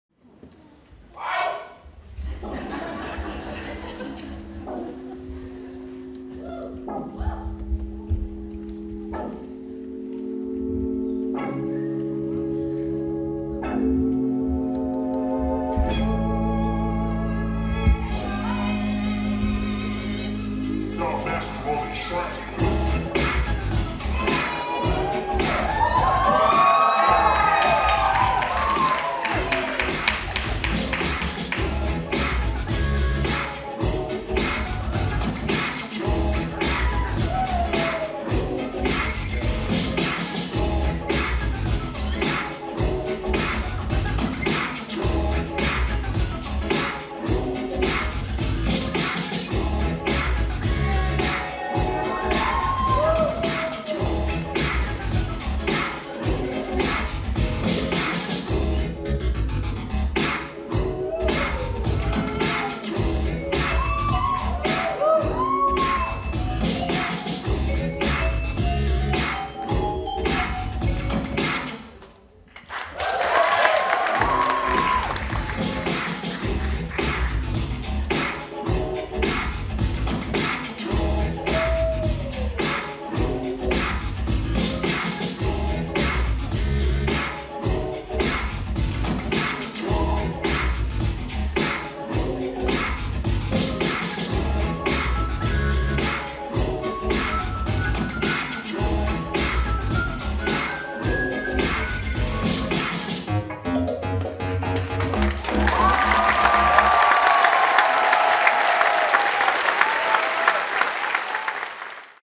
Electric Boogaloo Showcase
My electric boogaloo showcase in Vienna on 16 June 2022 (first time since 2019 and the pandemic in between).